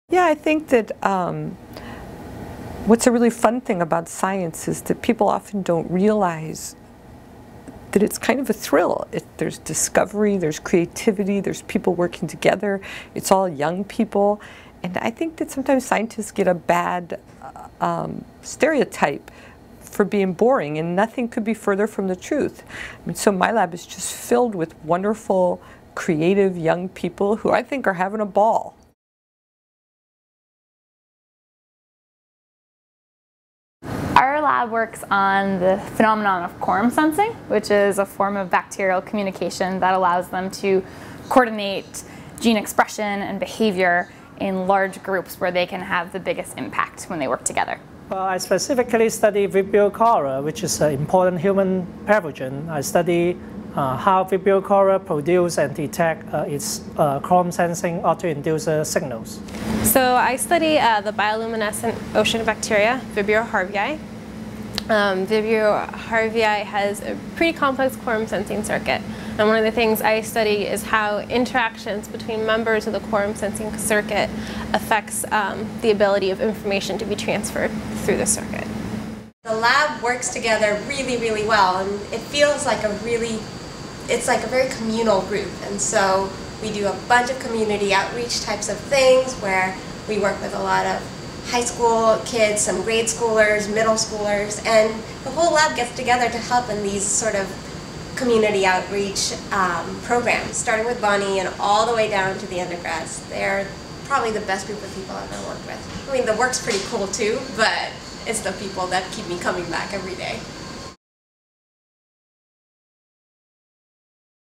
The Bassler Lab
Bonnie Bassler's laboratory at Princeton University studies how bacteria talk to each other through a process called "quorum sensing."